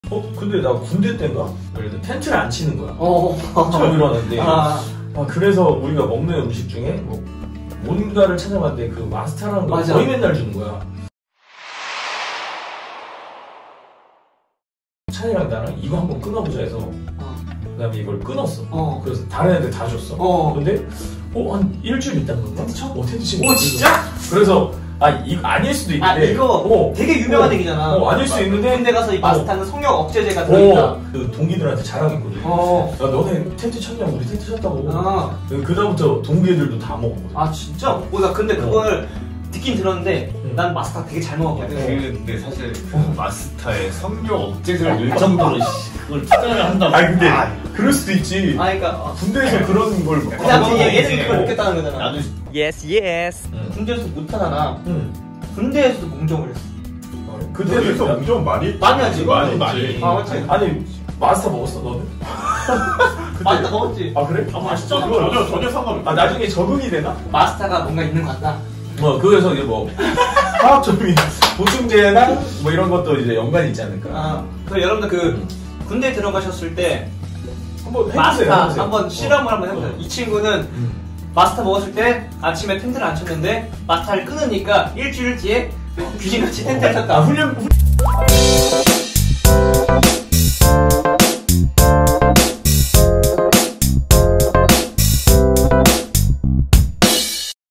군대에서 맛스타를 끊었더니 생긴 놀라운 변화 !?
앗 안녕하세요 ‘라룩’입니다!

‘맛스타’는 군대에서 자주 제공해주는 음료수 입니다
헌대 이 음료수에 성욕억제제가 들어있다는 학계에 정설이...

촬영을 도와준 나의 식구들
YKfamily 무한 사랑과 감사를 전합니다.